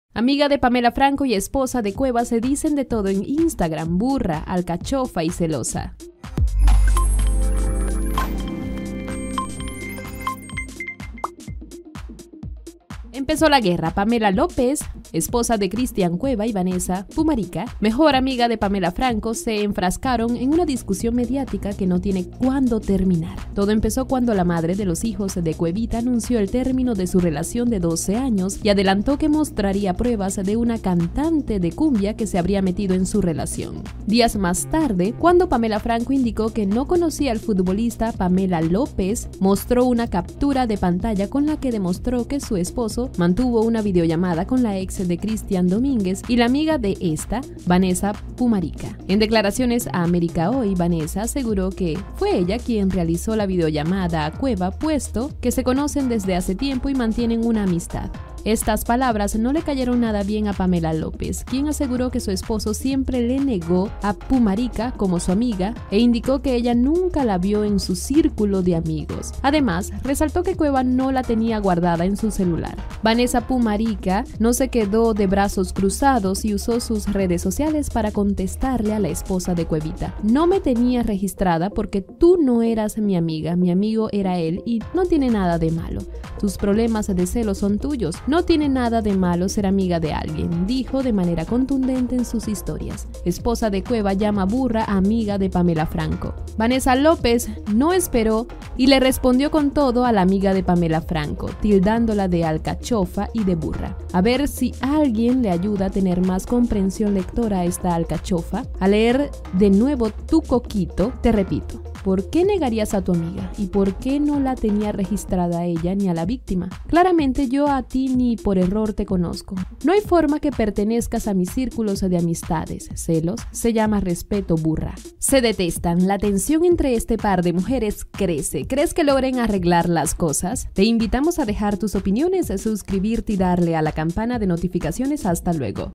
¡Burra y Alcachofa! Pamela López destruy3 a amiga de Pamela franco x infidelidad de Christian Cueva
Amiga de Pamela Franco y esposa de Cueva se dicen de todo en Instagram: ‘Burra’, ‘alcachofa’ y ‘celosa’
Vanessa Pumarica le dijo a Pamela López que su problema son los celos, mientras que la esposa de Cuevita la tildó de ‘alcachofa’ y ‘burra’.